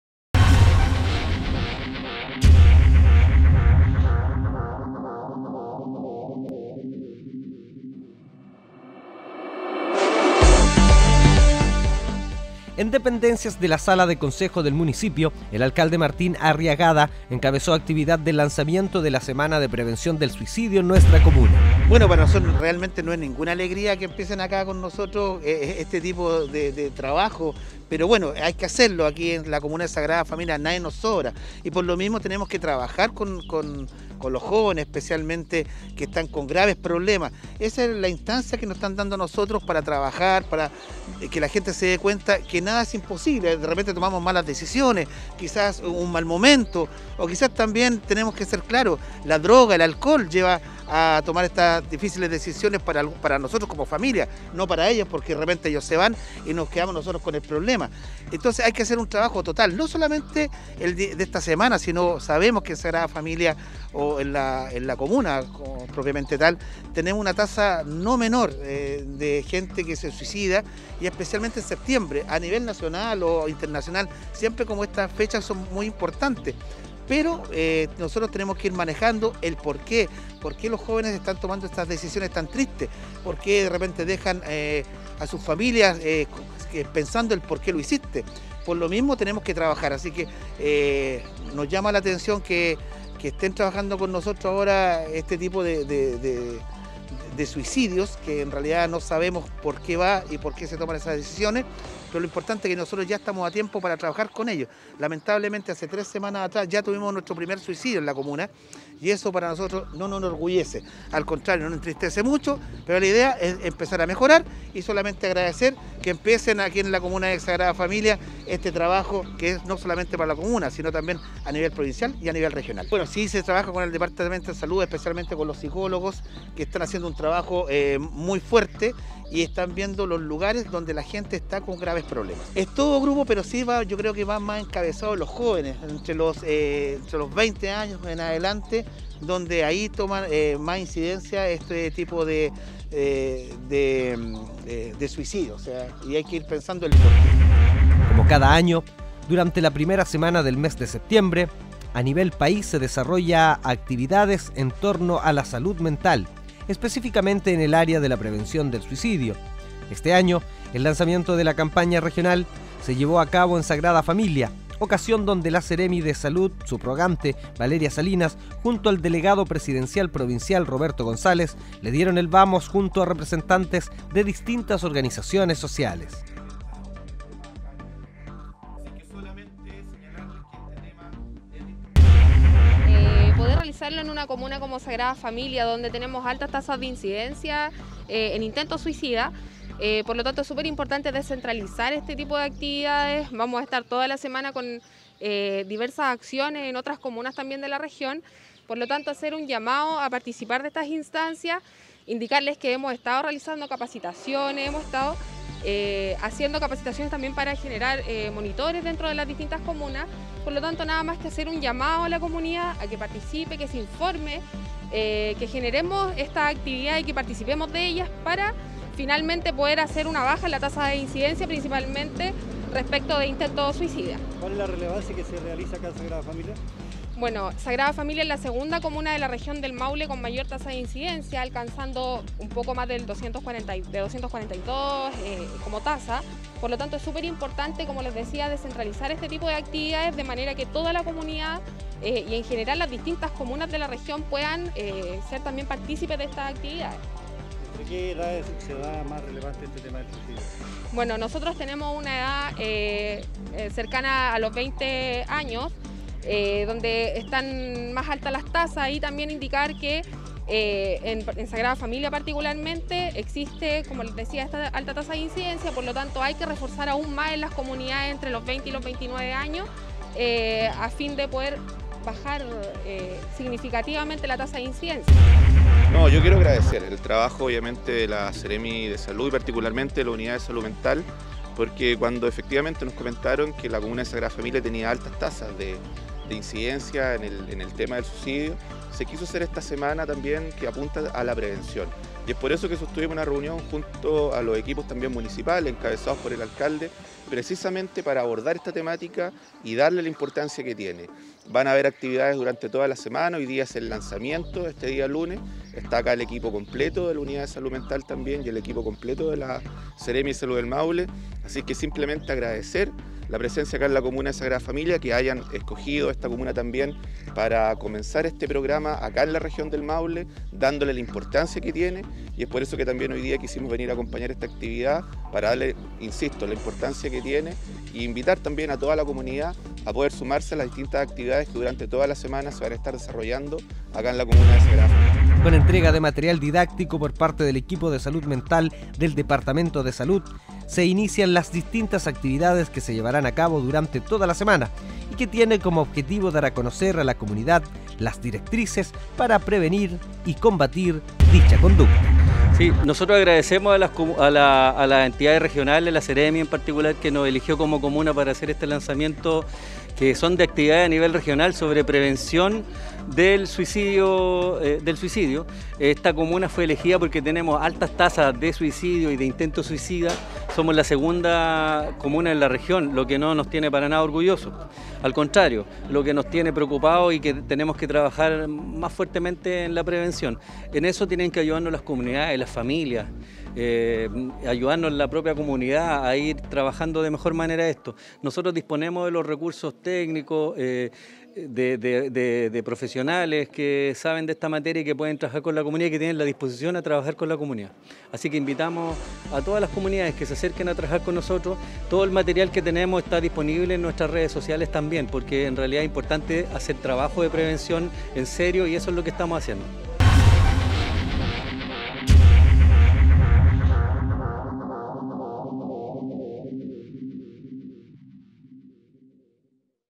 Lanzamiento semana Prevención del Suicidio
El AlcaldeMartínArriagada encabezó actividad de lanzamiento, de la semana de la prevención del suicidio en nuestra comuna, Junto a la Seremi de Salud (s), Valeria Salinas, el Delegado Presidencial Provincial, Roberto González, y representantes de distintas organizaciones sociales.